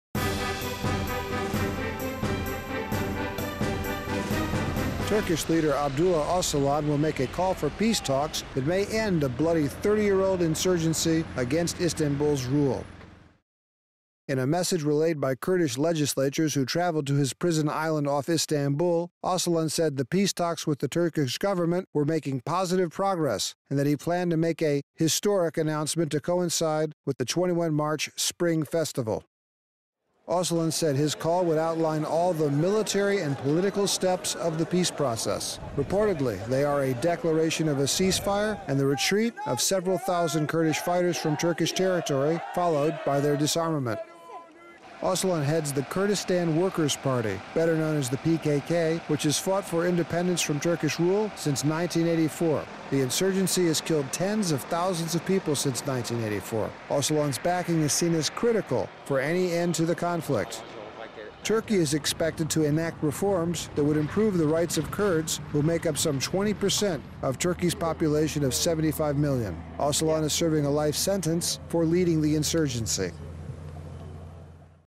Kurdish leader to make peace call: Abdullah Ocalan moves to end 30-year Turkish conflict
Turkish rebel leader Abdullah Ocalan will make a call for peace talks that may end a bloody 30-year-old insurgency against Istanbul's rule.